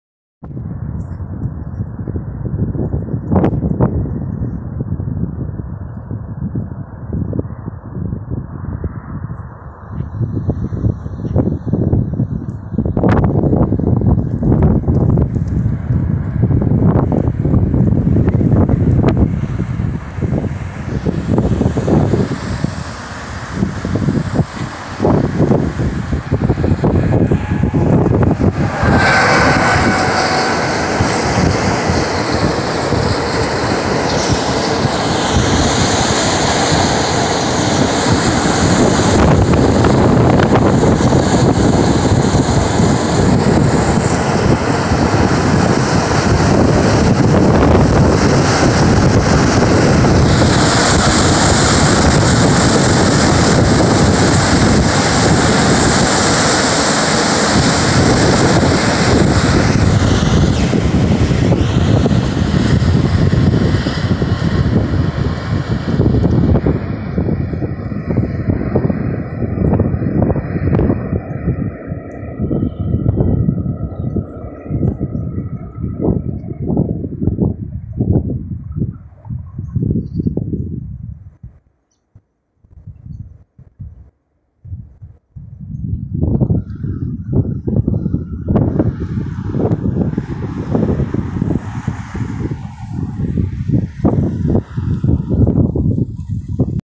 Locomotora 251 con Pecovasa en el parque JC1.
Mitsubishi electric 251 series locomotive with Pecovasa car-carrier train pass Juan Carlos 1 park in Madrid.